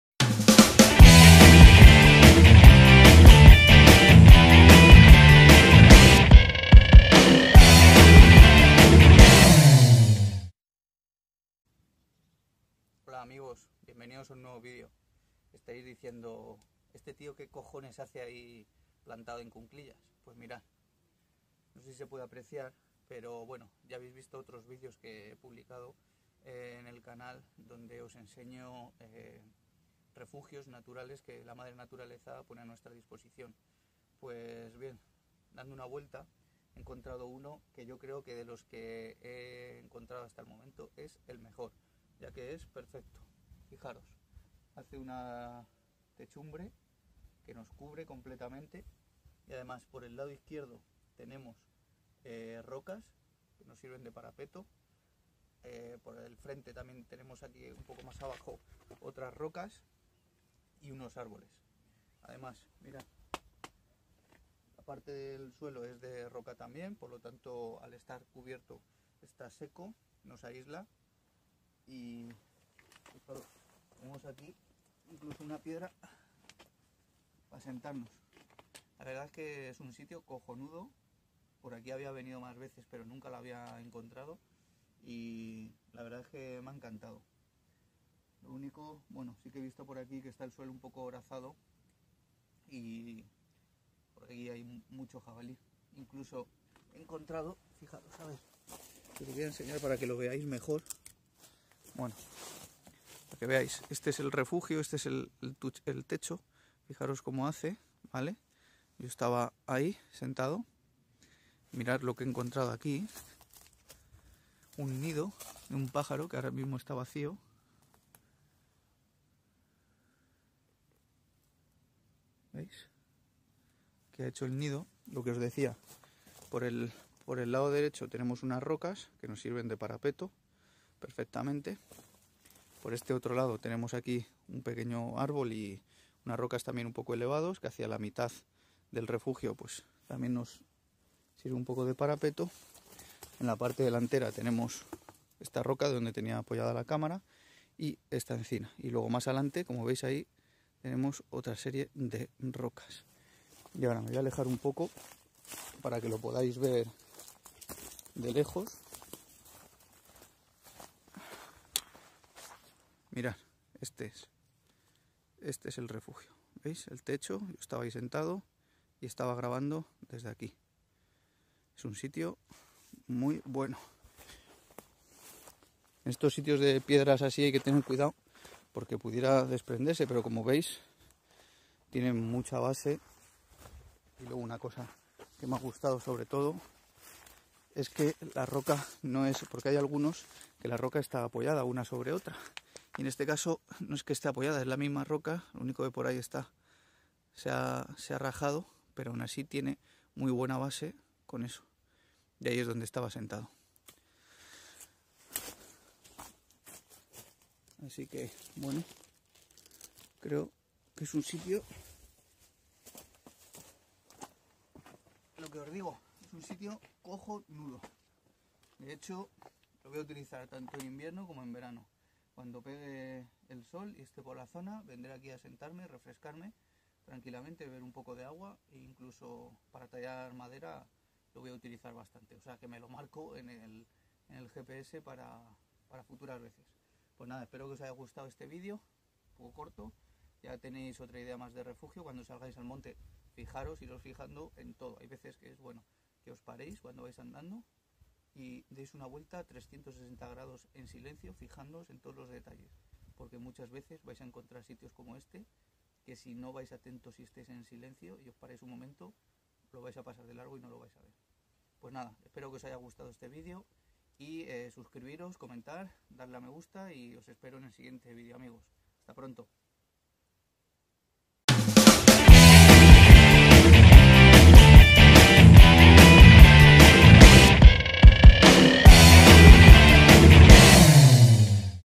El Mejor Refugio de Supervivencia con Materiales Naturales
En este video vemos un refugio natural de SUPERVIVENCIA o BUSHCRAFT con materiales naturales que nos ofrece la Madre Naturaleza para cobijarnos o pasar la noche en caso de necisidad o VIVAC. Estos refugios los podemos encontrar en el monte pero muchas veces pasan desapercibidos a nuestros ojos.